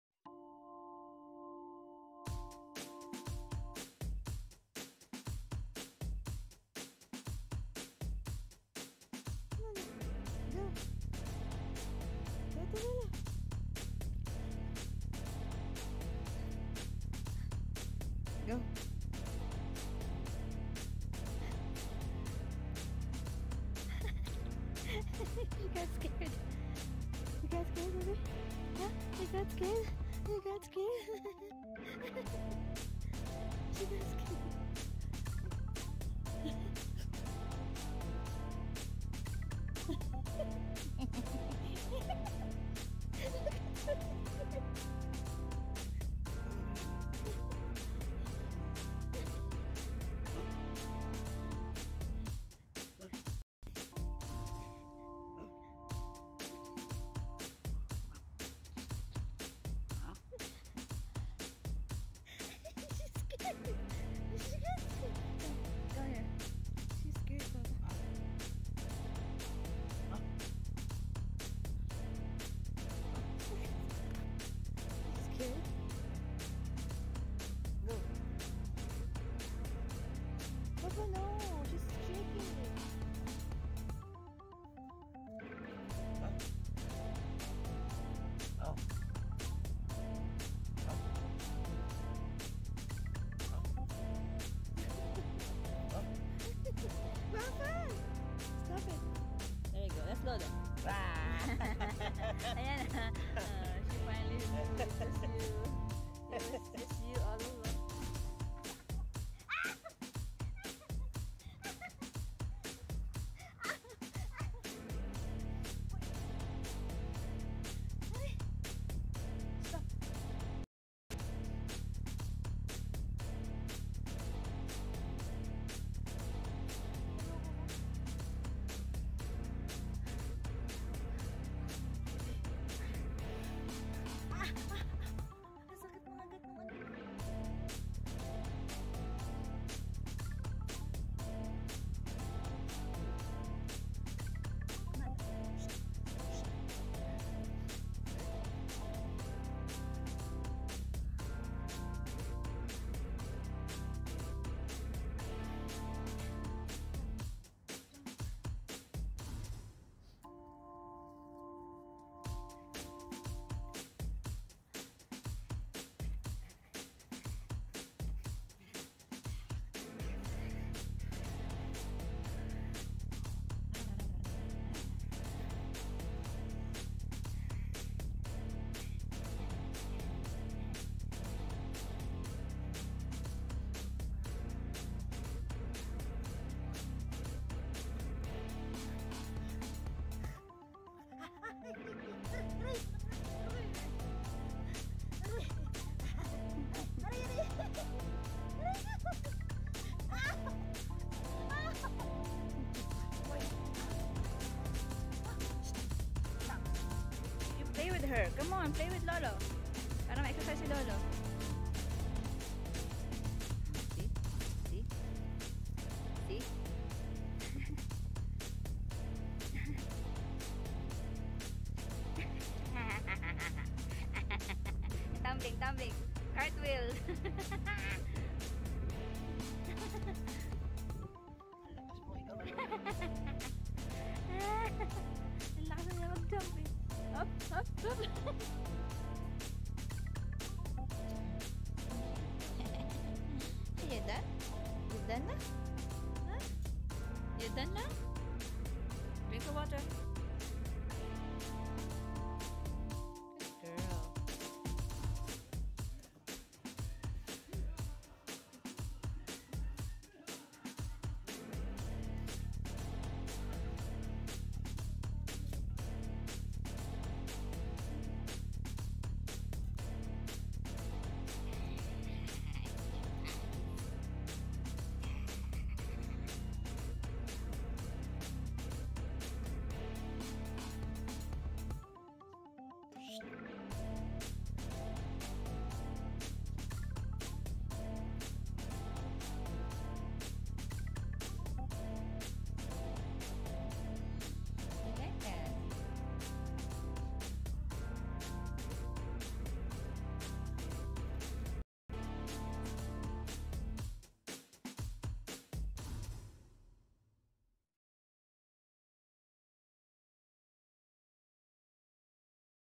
Playtime with our baby girl
She grows bigger everyday. it's such a wonderful blessing to have her in our lives. she make each day so bright and cheerful